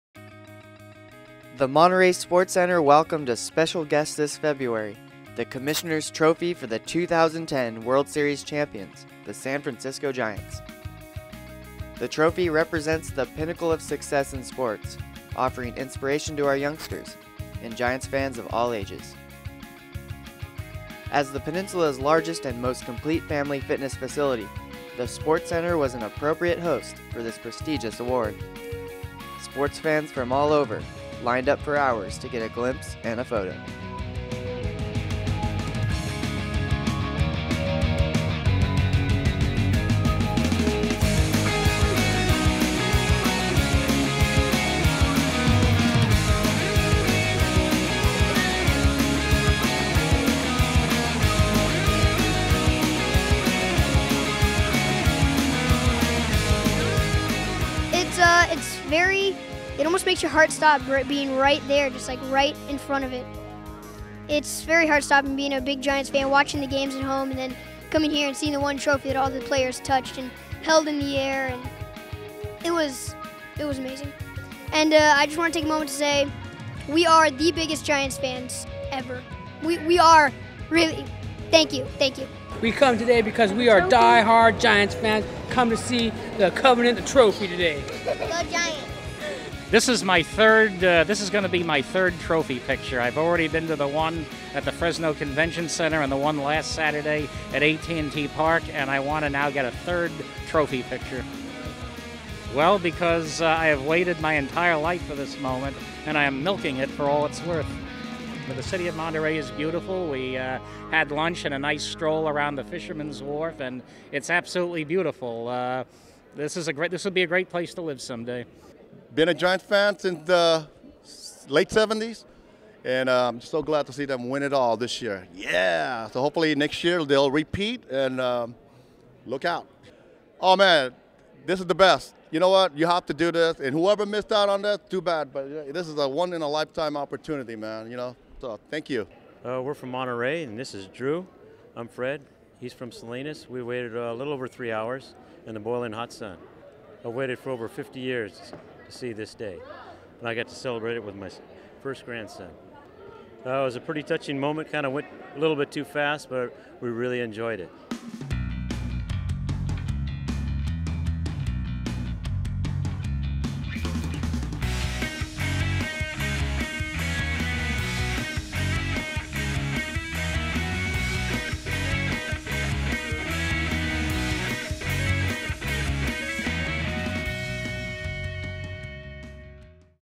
SF Giants Trophy Visits Monterey
The City of Monterey gets a special visit from the San Francisco Giants' World Series Championship Trophy.